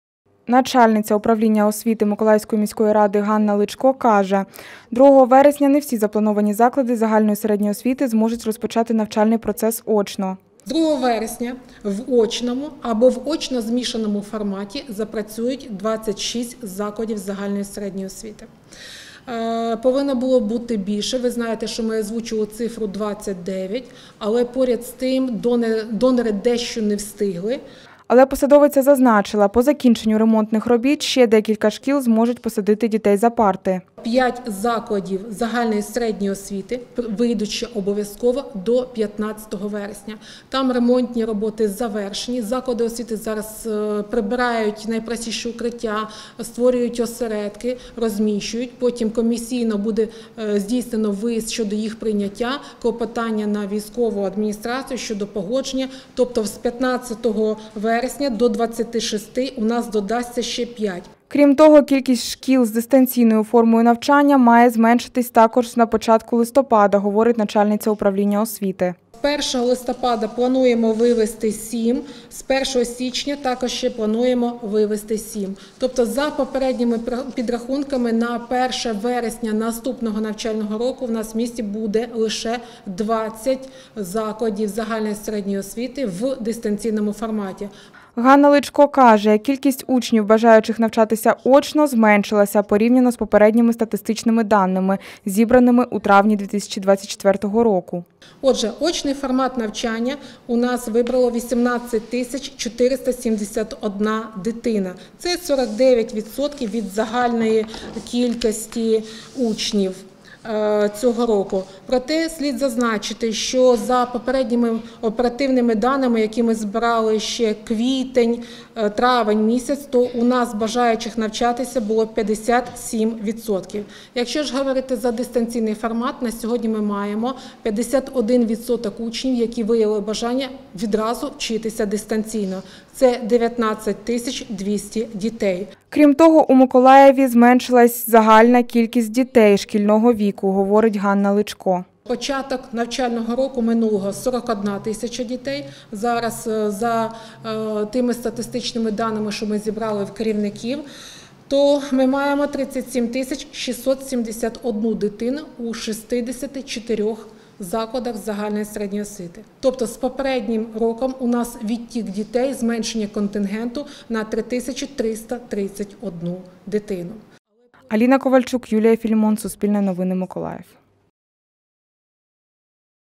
У Миколаєві з другого вересня 26 шкіл працюватимуть очно й у змішаному форматах
У Миколаєві другого вересня 2024 року 26 закладів загальної середньої освіти запрацюють в очному та змішаному форматах. На початку листопада кількість шкіл з дистанційною формою має зменшитись. Загалом навчання в офлайн-режимі обрало 18 тисяч 471 дитини, що становить 49 відсотків від загальної кількості учнів в цьому році.

Авторка: Аліна Ковальчук
Оператор: Юлія Філімон